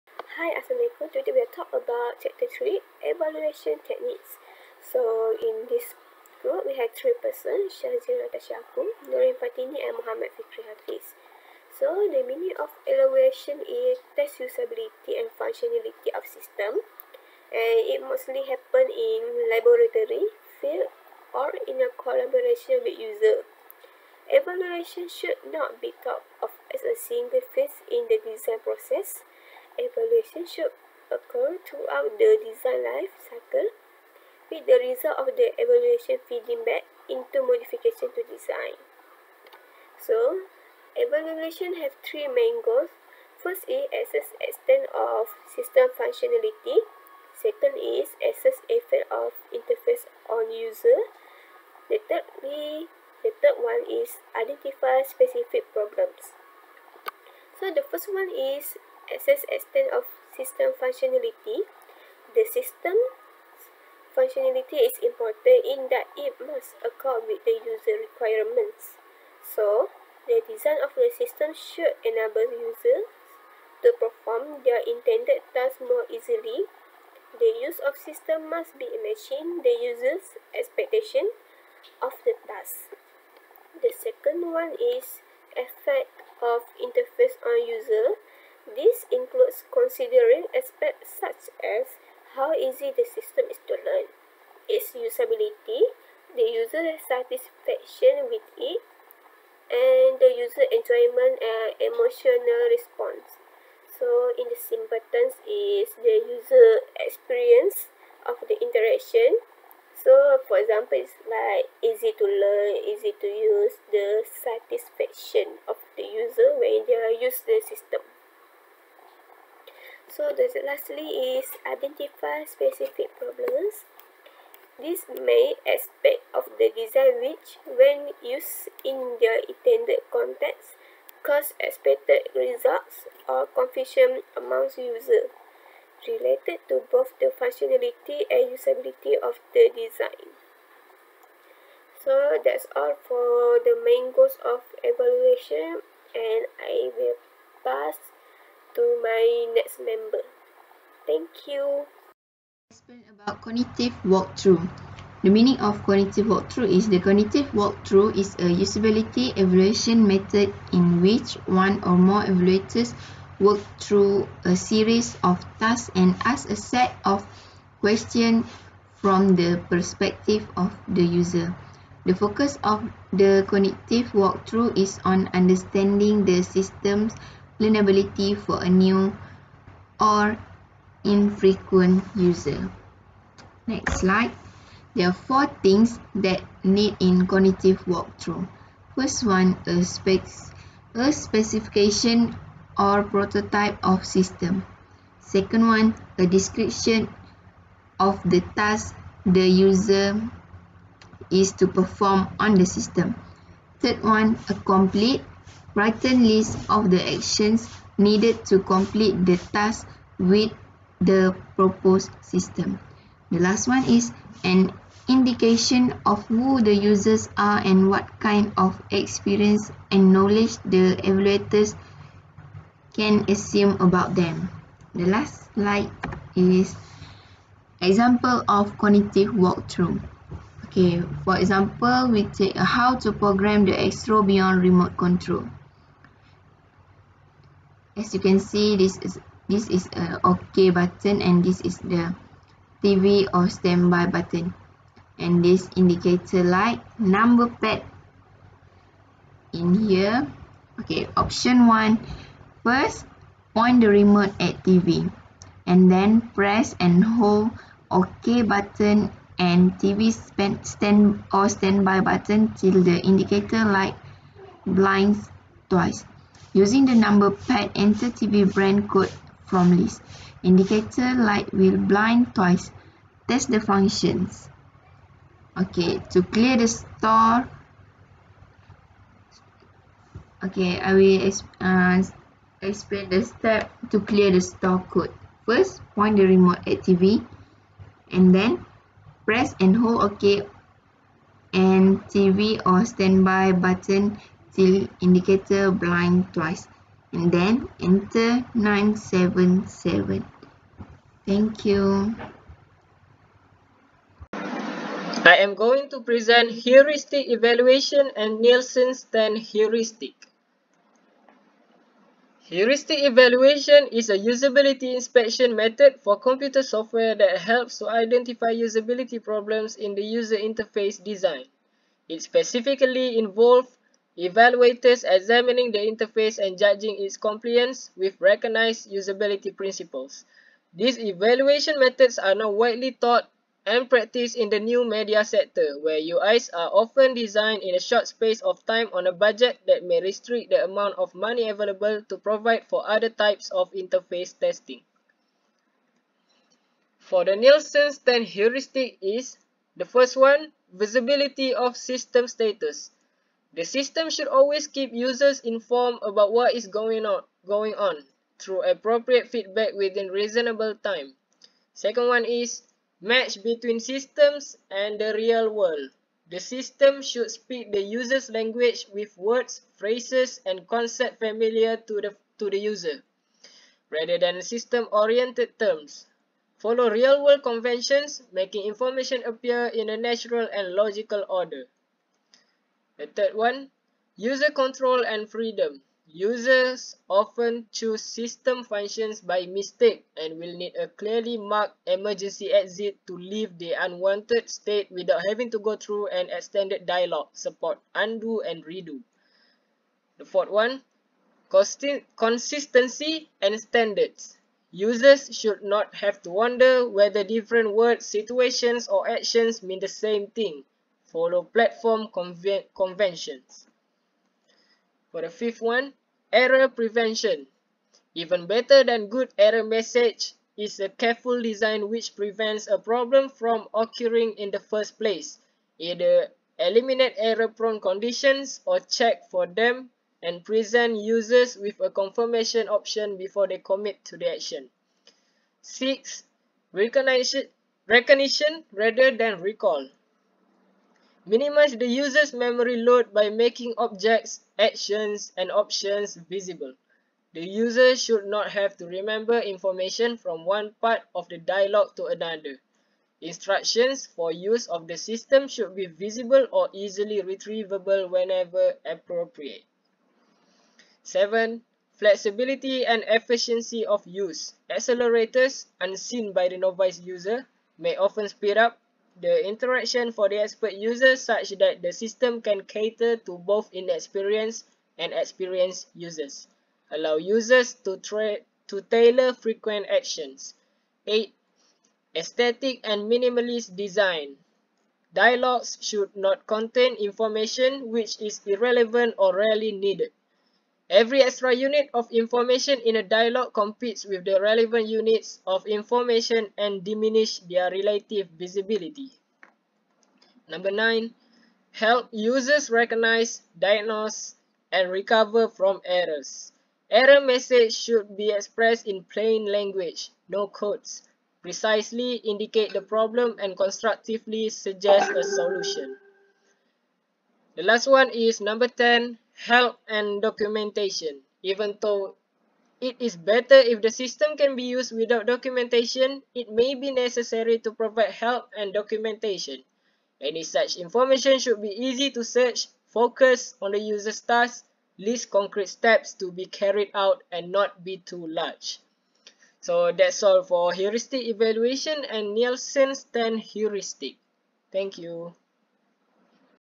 Evaluation techniques-HCI
Assalamualaikum and good evening to everyone. We are from DDT3D will present about evaluation techniques. Please enjoy it. Thankyou

Group members:
1- Shazeera natassha khoo
2- Nurin fatini
3- Muhammad fikri hafiz